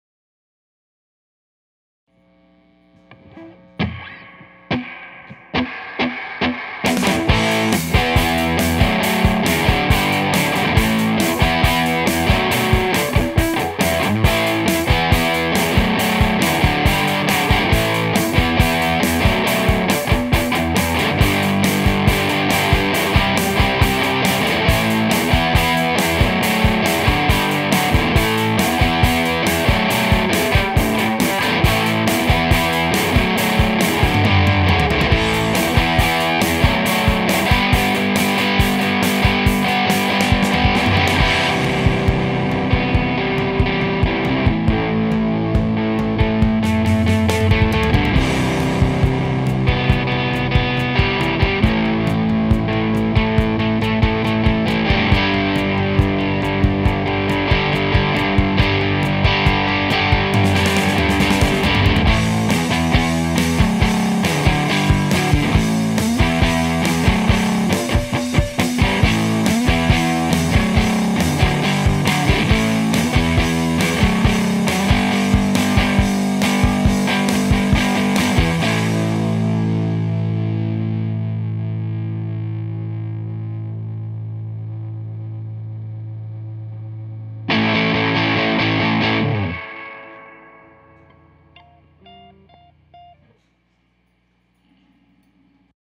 Springocalypse | Suhr Standard / Apocalypse Fuzz / White Whale Spring Reverb / Supro Keeley Custom
We live in an amazing world where you can send one of the classiest spring reverbs into an apocalyptic fuzz and get a sound this great. The custom Suhr Standard and Supro amp sure doesn't hurt either. Check out all this great gear at distinctiveguitar.com.